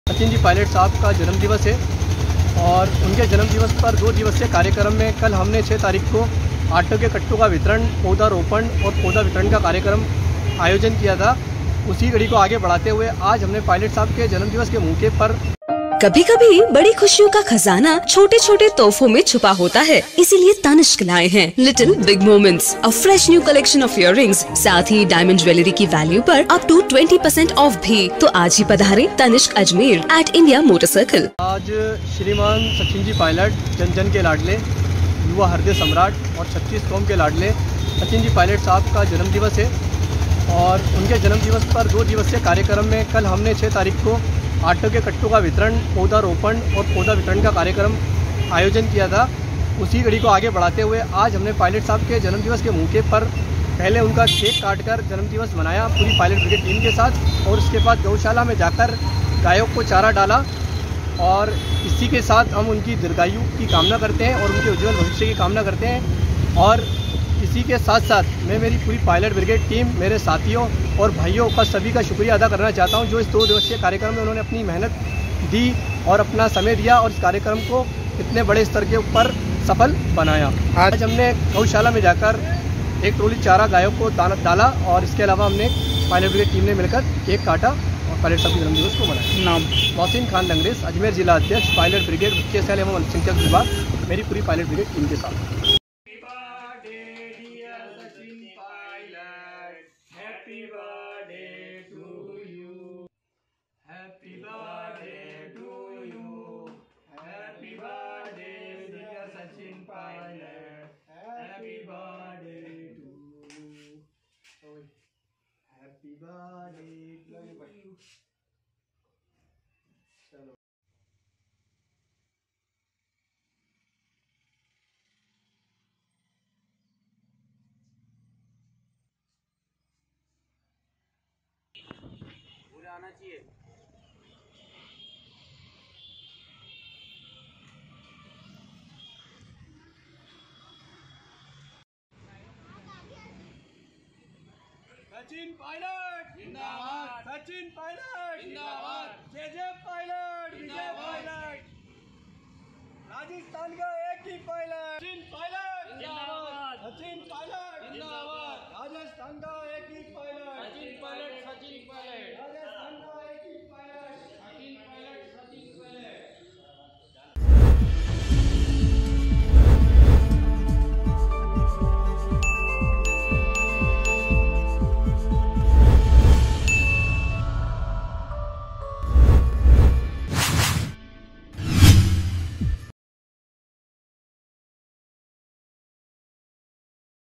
Ajmer | सचिन पायलट का जन्मदिवस आज | MTTV INDIA
सचिन पायलट का जन्मदिवस आज
पायलट ब्रिगेड टीम ने जन्मदिवस के उपलक्ष में गौशाला में एक ट्रोली चारा किया वितरित